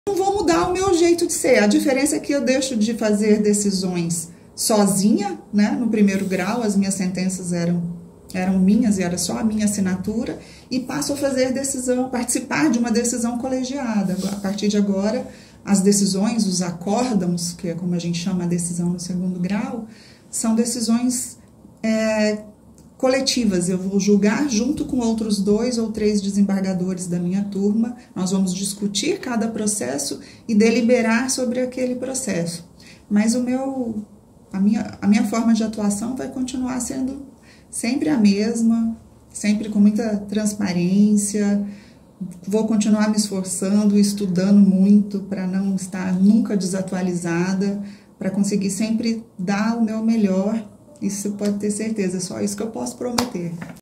Neste ano, Eleonora Lacerda completa trinta anos de trabalho prestado ao TRT MT
Nova desembargadora do TRT-23, fala da trajetória até assumir a Justiça do Trabalho no estado